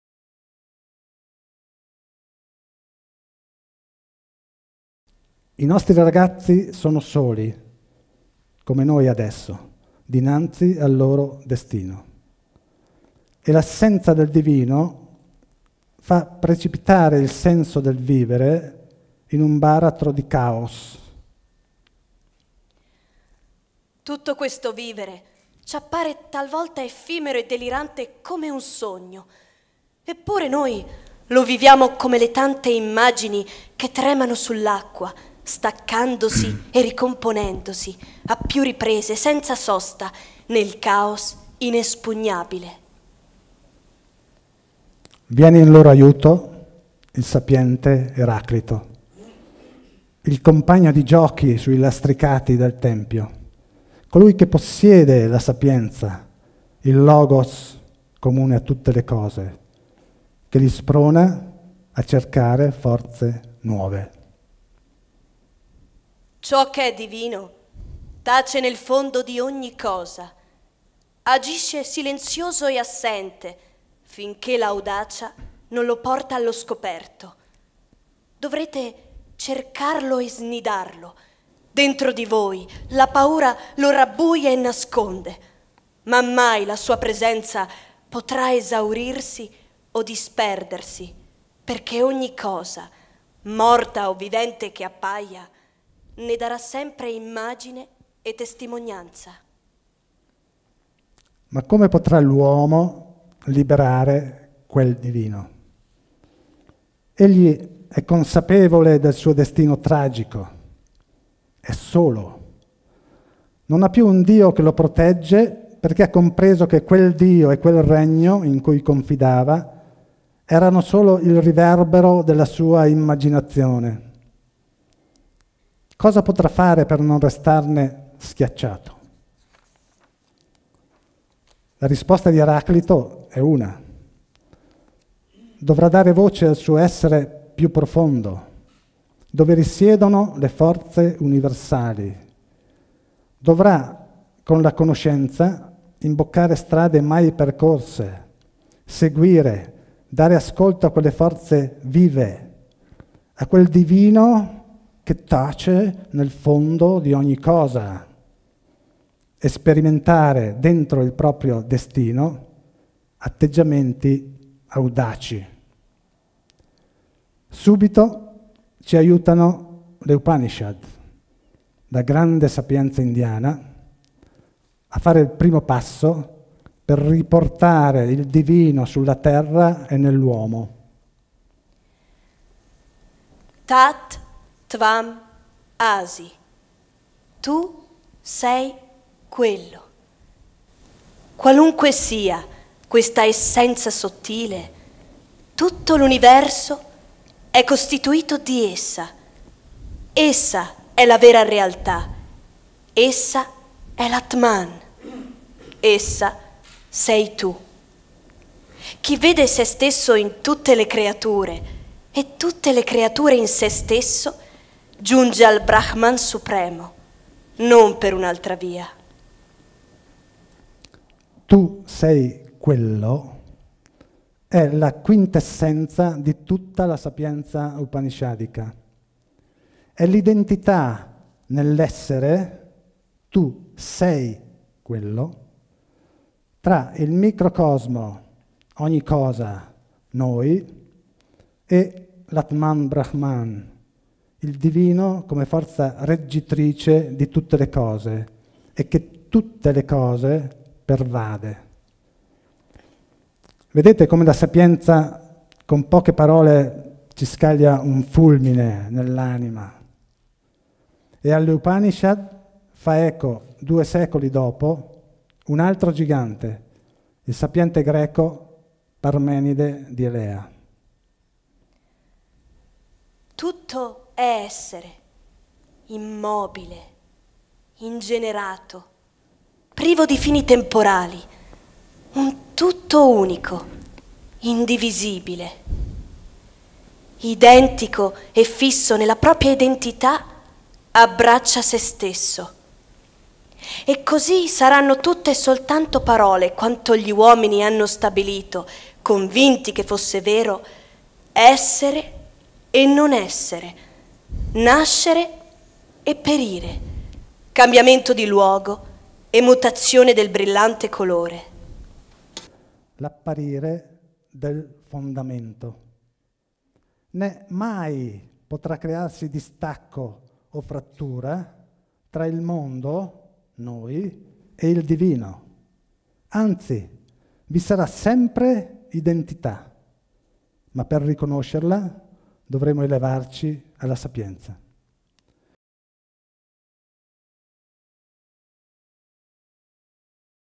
ENRICO ISCHIA - Dal Caos alla Sapienza
L' ARCIPELAGO L'APPARIRE DEL FONDAMENTO di  ENRICO ISCHIA. Tratto dalla presentazione del libro del 6 aprile 2019 con Angelo Tonelli. Dalle Upanishad al Bhagavadgita, da Eraclito a Schopenhauer e Nietzche